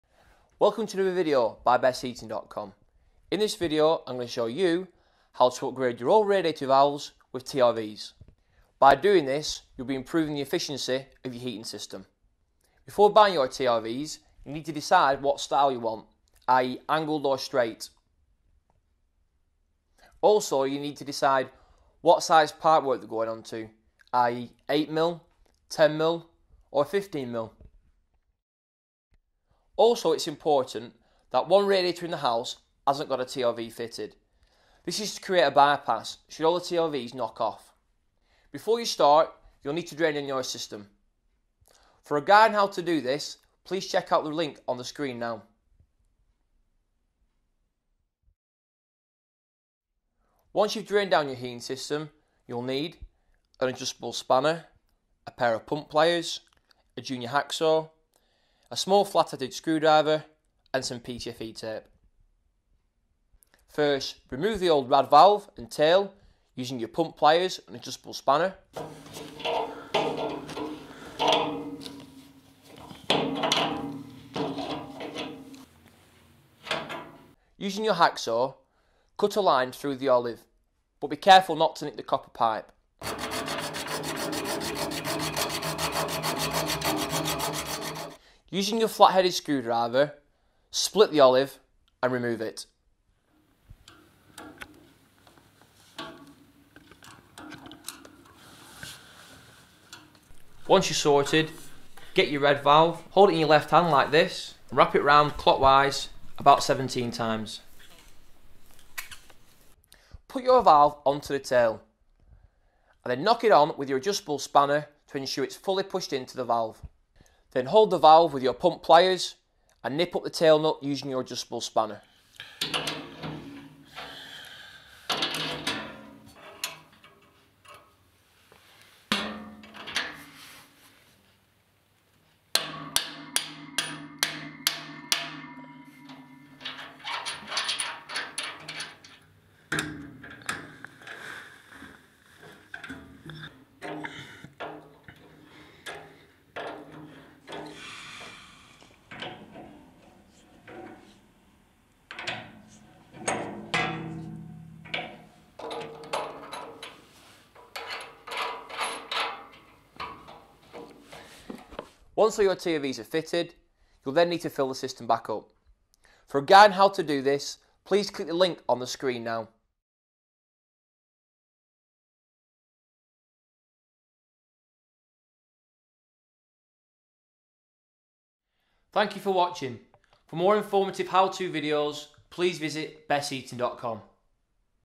How To Install TRV's To A Radiator | BestHeating
Installing TRV's will make your heating system more efficient.

BestHeating's resident plumber will take you through all the steps on installing TRV's in this quick and easy to follow video.

For this, you'll need:
- An Adjustable Spanner
- 1 Pair of Pump Pliers
- A Junior Hacksaw
- Flat Head Screwdriver
- PTFE Tape

For more tips, tricks and how-to's please visit our Advice Centre

Stay Safe & Happy Heating